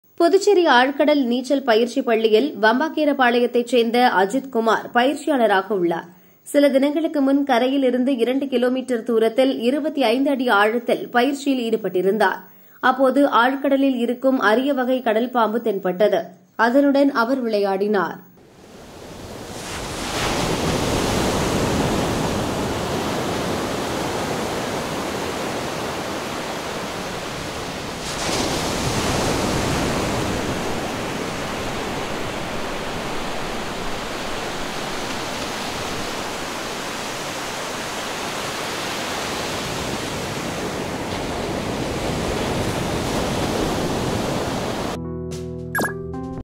கடல் பாம்புடன் விளையாடும் ஆழ்கடல் நீச்சல் பயிற்சியாளர்
கடல் பாம்புடன் விளையாடும்
ஆழ்கடல் நீச்சல் பயிற்சியாளர்Puducherry